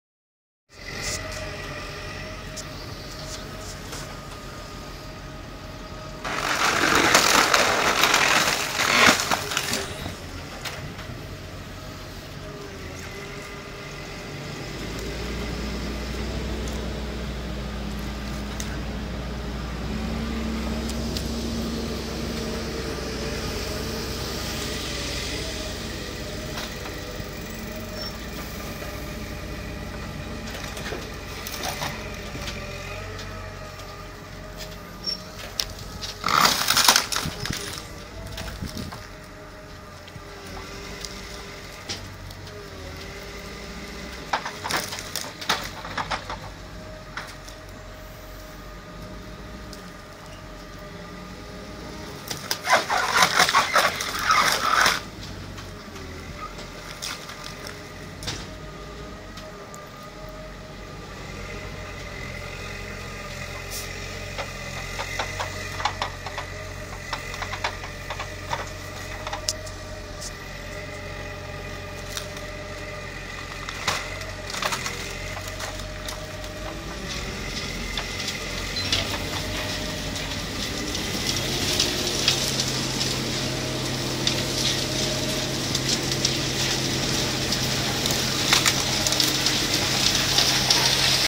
Cat M313C met woodcracker
Caterpilar M313C kraakt hout met een Woodcracker